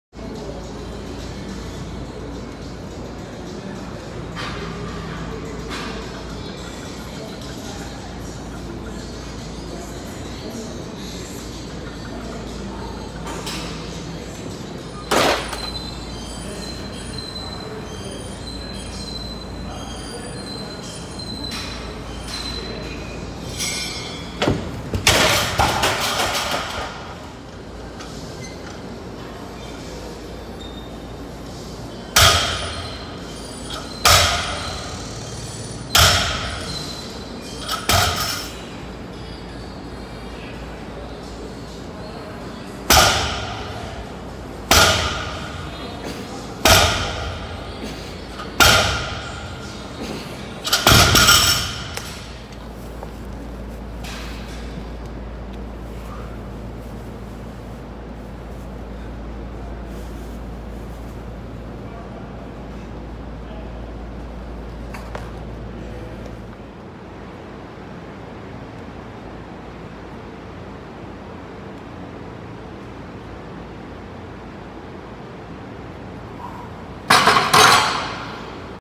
30min Workout
Last night 30min workout. Lost balance on my 8th rep. All is good. To redeem- focused on deadlift and incline